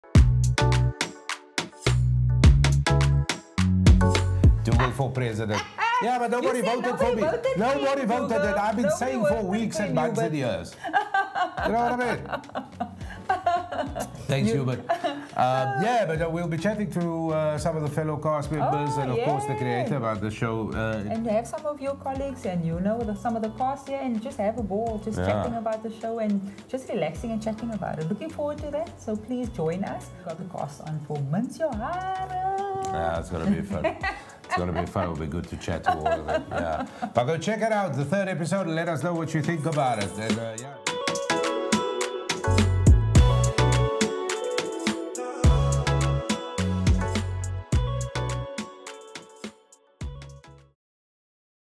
Mahala Monday - "Mince Jou Hare" interview trailer
🌟 Join us for the latest episode of MahalaMonday! 🌟

🎥 This week, we’re chatting to the hilarious Shimmy Isaacs and the talented cast of the Showmax original "Mince Jou Hare".

Tune in to discover:
✨ Shimmy Isaacs's laugh-out-loud sitcom about a factory worker who opens a hair salon in her home.
✨ The small-town comedy inspired by Worcester in the Western Cape.
✨ How Frieda’s new salon shakes up her family, neighbours, and clients.

Meet the creative minds behind the show:
🎬 Written and co-produced by award-winning comedian Shimmy Isaacs, based on her mother's life.
🎬 Directed by David Isaacs, co-creator of Joe Barber and G.I.L., and director of Swartwater and Sara Se Geheim.
🎬 Produced by Roberta Durrant of Penguin Films, known for International Emmy nominees Stokvel and Home Affairs, as well as classic SA comedies like Madam & Eve.

Grab your coffee and be part of the conversation! 🗣️✨